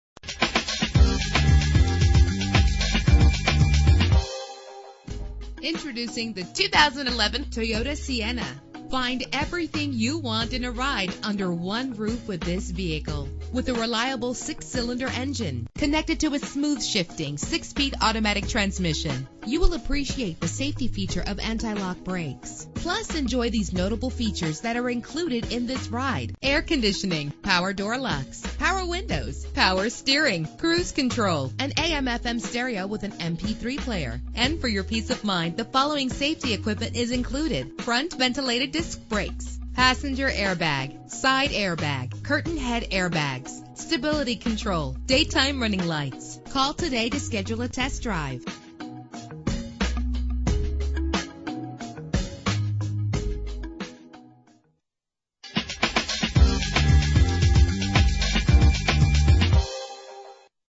2011 Toyota Sienna - Danvers MA
Year: 2011
Make: Toyota
Model: Sienna
Trim: LE
Engine: 3.5 liter 6 cylinder 24 valve
Transmission: 6-Speed Automatic
Color: Silver Sky Metallic
Mileage: 11
Add